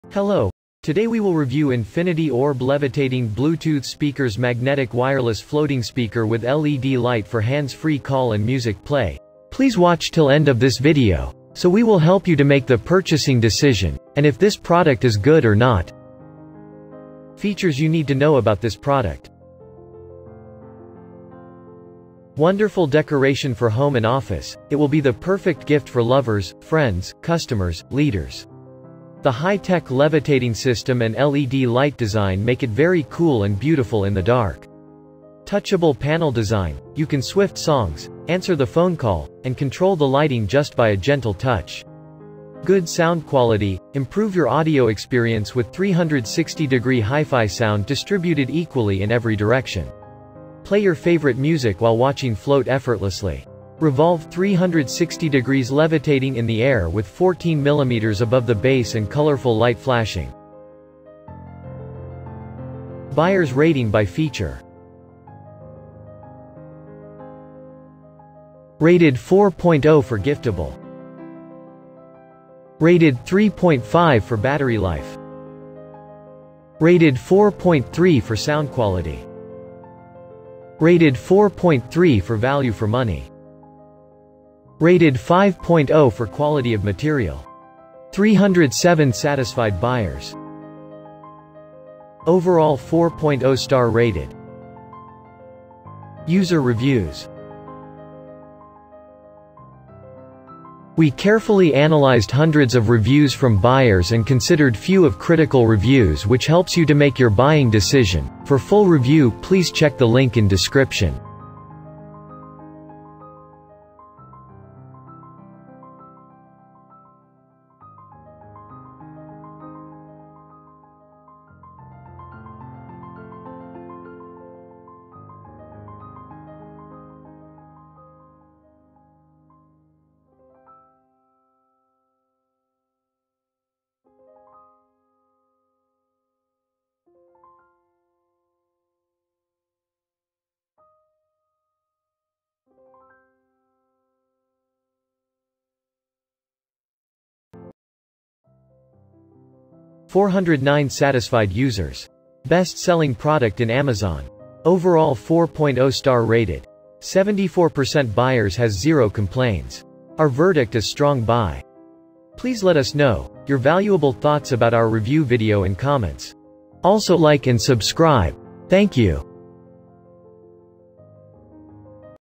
Review Of Infinity Orb Levitating Bluetooth Speakers Magnetic Wireless Floating Speaker With Led...
Infinity Orb Levitating Bluetooth Speakers Magnetic Wireless Floating Speaker With Led Light For Hands-Free Call And Music Play

✔ 338 Satisfied Users.
✔ Best Selling Product In Amazon.
✔ Overall 4.0 Star Rated.
✔ 74% Buyers Has Zero Complains.
✔ Our Verdict Is Highly Recommended To Buy.

Timeline

✔ 00:00:21 - Features
✔ 00:01:23 - Feature Wise Star Rating by buyers
✔ 00:01:40 - Customer Testimonials
✔ 00:02:29 - Our Verdict